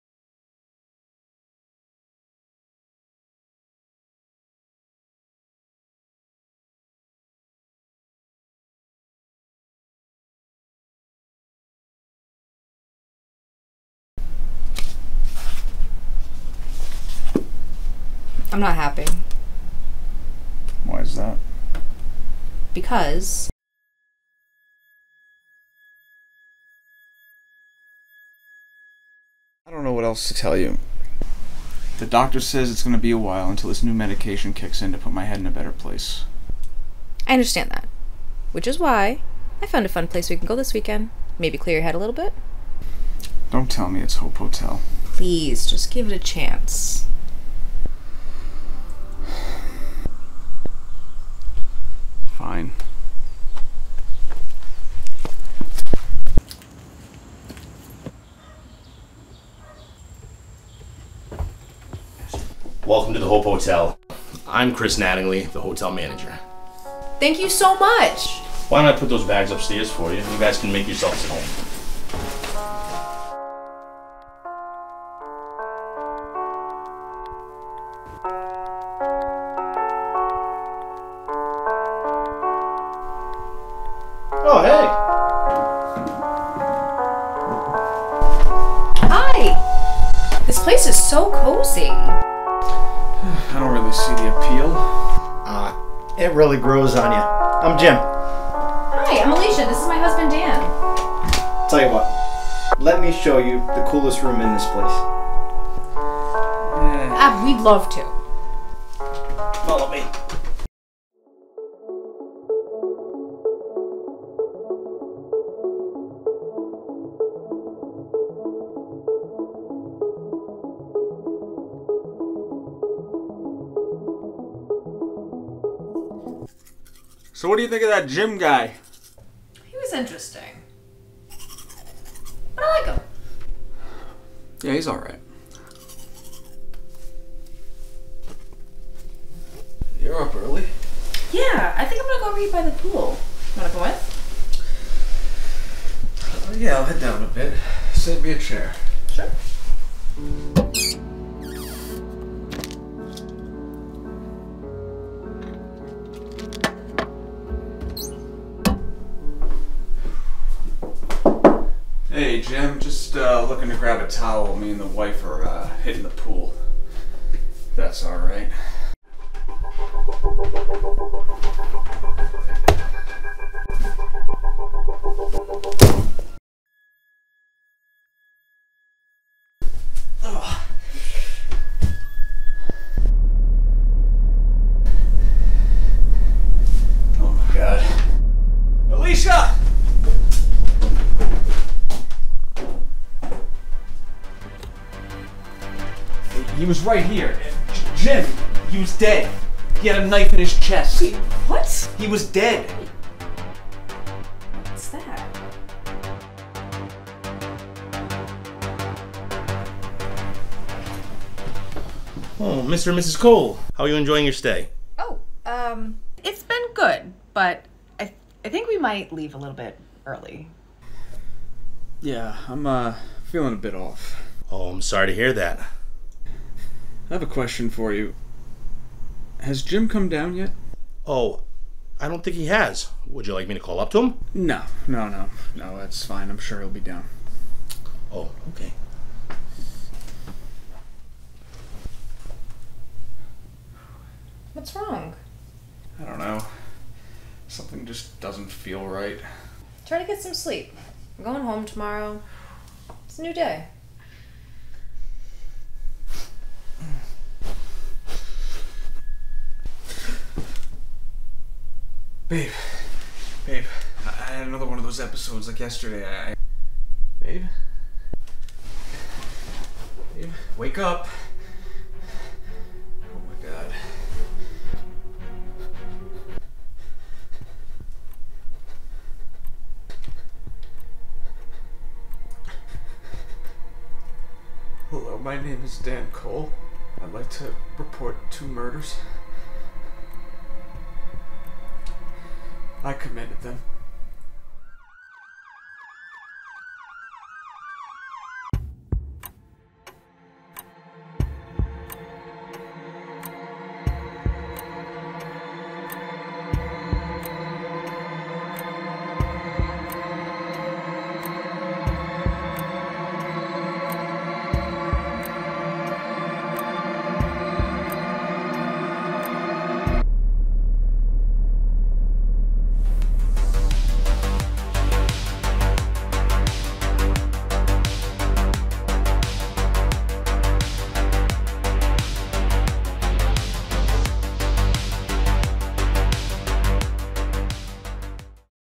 Hope Hotel
Our 2022 Submission into the 48 Hour Film Festival!

Special thanks to MilitantCow for his music
Jackie + Alex for their house!